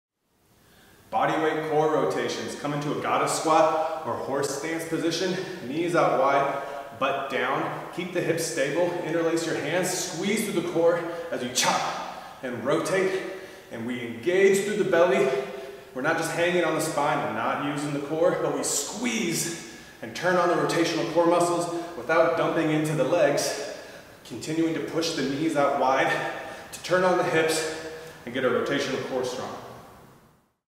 Bodyweight Core Rotations
Squat into a wide horse stance position with your knees out wide and your shoulders stacked on top of your hips. Clasp your hands into fists together and rotate through the ribcage as you anchor your pelvis and low back into position.